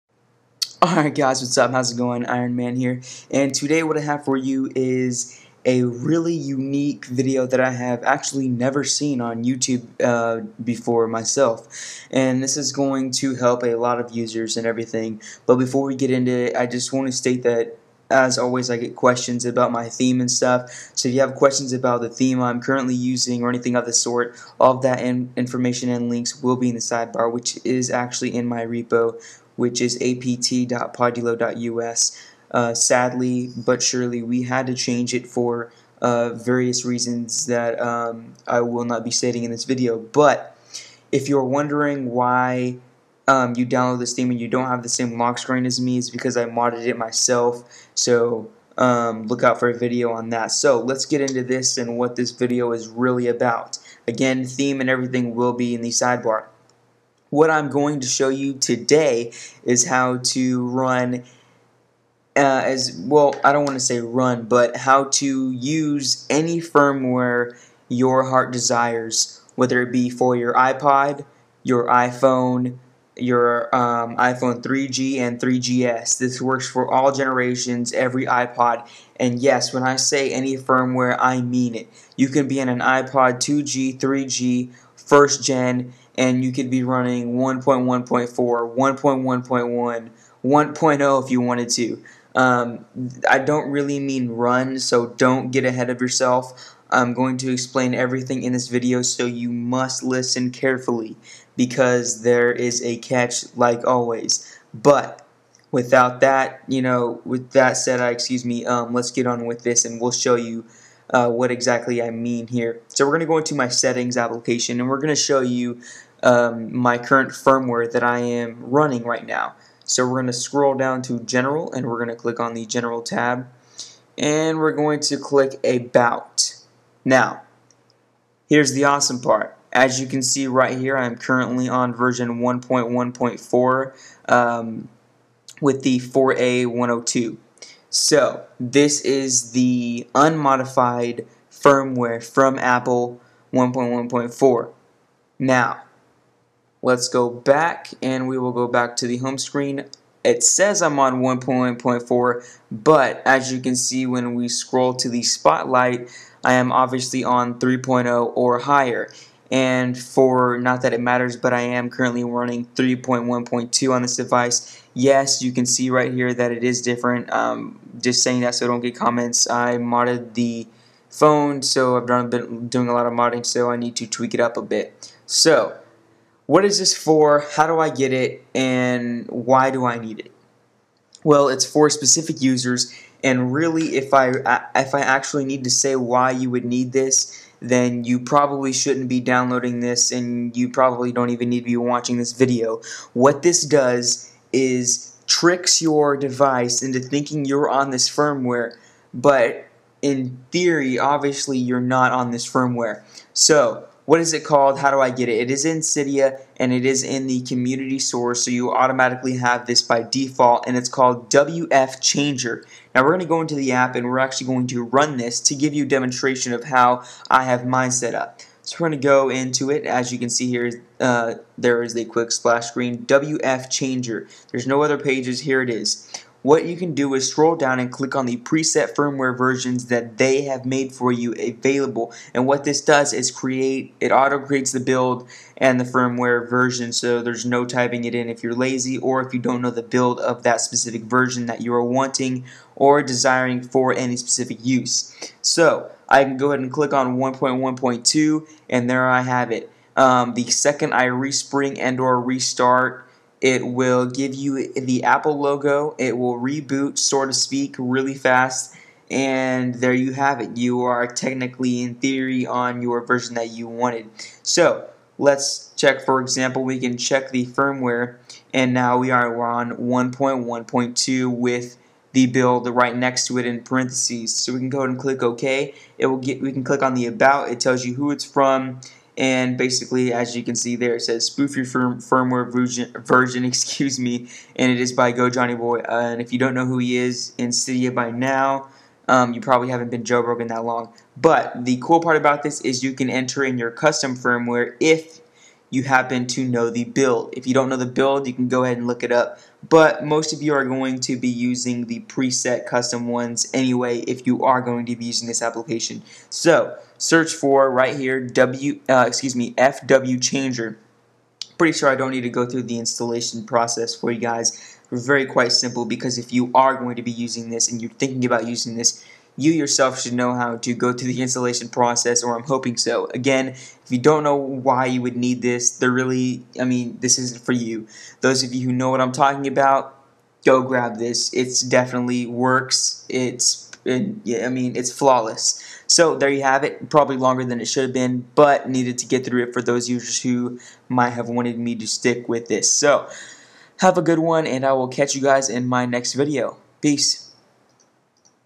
Easily Change iPhone/iPod Touch 2G/3G/3GS Firmware With FWChanger
My current Theme: Matts Matte

Application used: FWChanger
Source: BigBoss

NOTE: This does NOT change your firmware completely, only modifies the plist to make your device THINK its on that specific firmware.